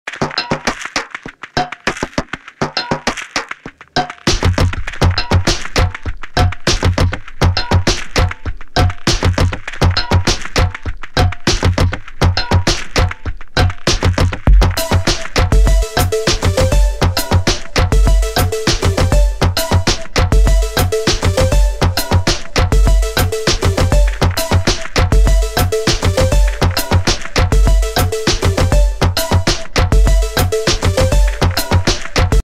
Hannah Indigo | LED Hoop Dance
Sneak preview footage feat. Hannah Indigo using Astral Atomic Evoke twin hula hoops.